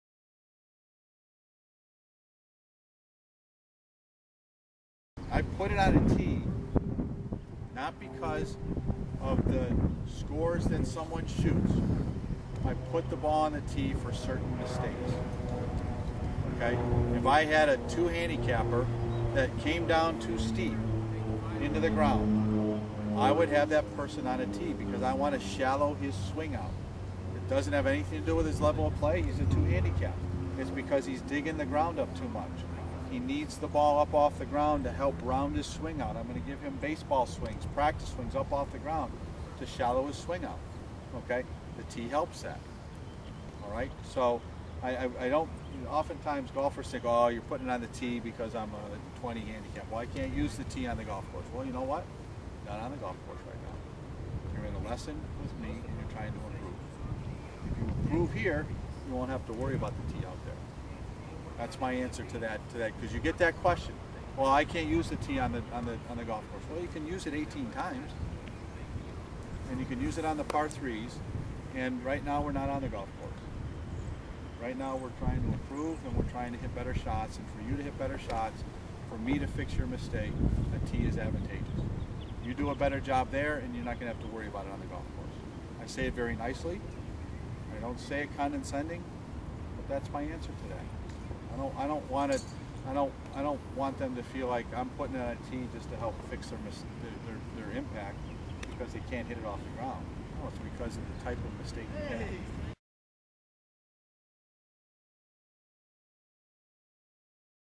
Coaching Champions - Correcting Swing Mistakes - When to Put the Ball on a Tee
An excerpt from one of my Coaching Champions seminars.  How putting the ball on a tee helps correct swing mistakes - even for the best players.